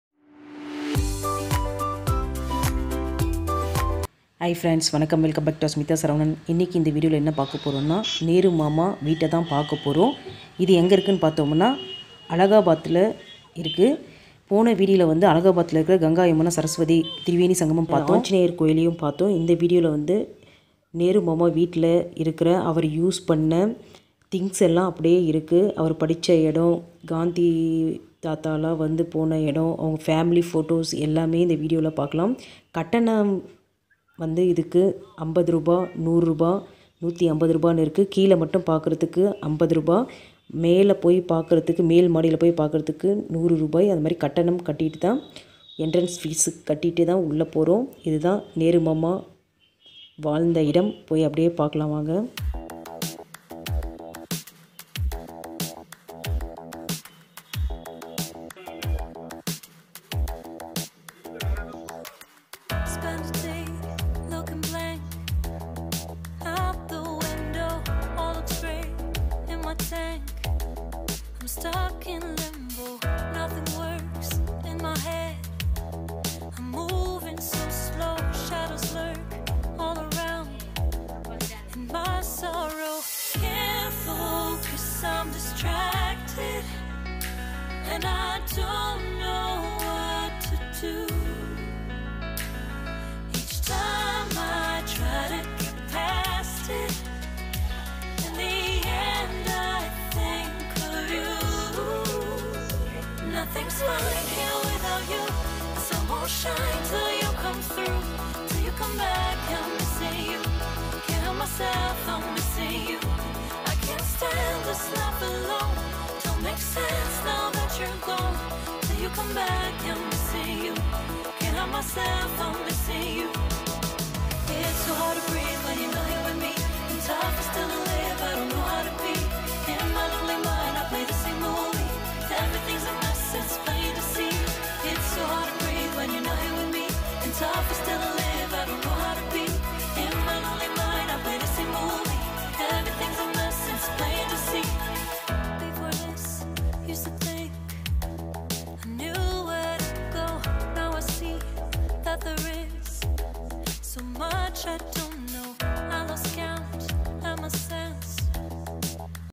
அலகாபாத்தில் உள்ள ஜவர்களால் நேரு மாமா வீட்டையும் அவர்களின் அறிய குடும்ப புகைப்படமும் பார்க்கலாமா🌹🌹🌹🥰
ஜவர்களால் நேரு மாமா வீட்டையும் அவர்கள் குடும்ப புகைப்படமும் பார்க்கலாமா🌹🌹🌹🥰

prayagraj vlog
children 's day
# ஜவர்கலால் நேரு
# நேரு மாமா
நேரு மாமா வீடு
மோதிலால் நேரு
நேரு மாமா புகைப்படம்